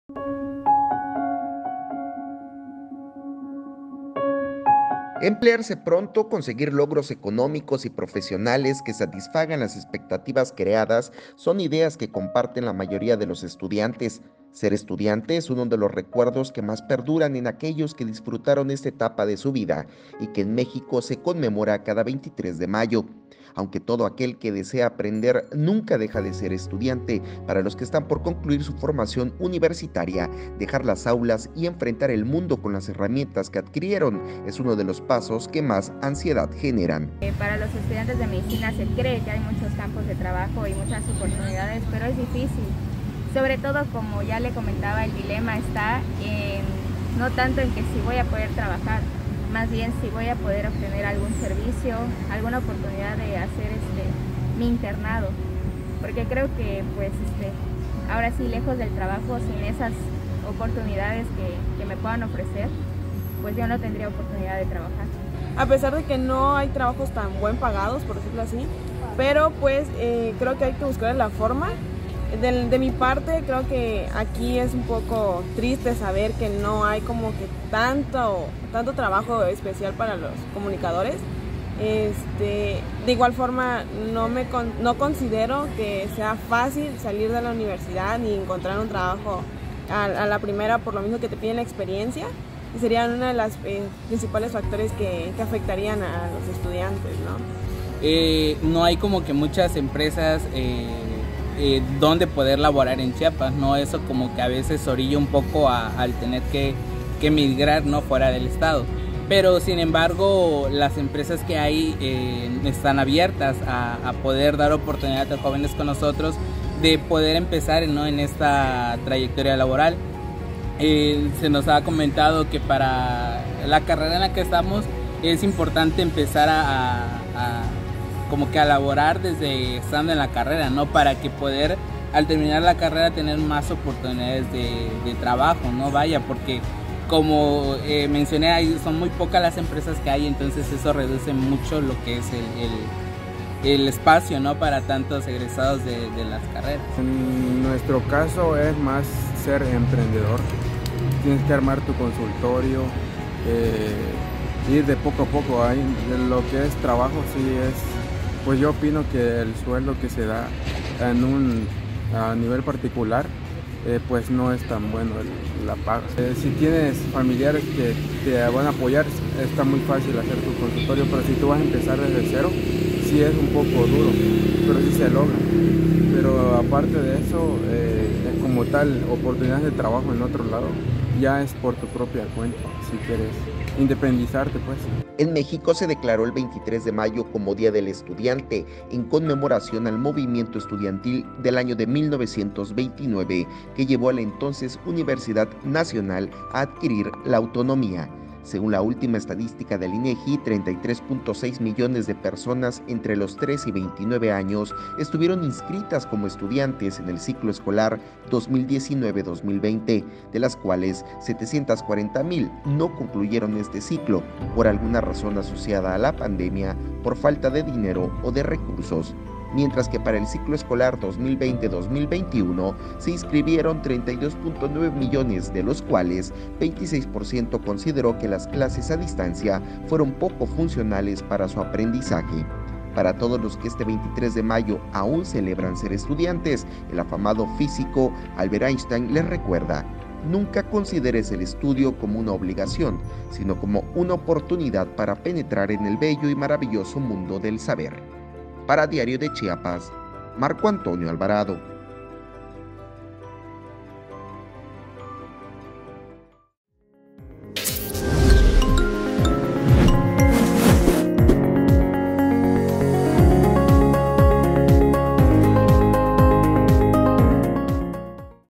Día del estudiante en México; más de 30 millones aún en las aulas
👨🏻‍🏫👨🏽‍🦱📄 Videorreportaje | Ser estudiante es uno de los recuerdos que más perduran en aquellos que disfrutaron esta etapa de su vida, y que en México se conmemora cada 23 de mayo.

🎙️Entérate de todos los detalles con Marco Alvarado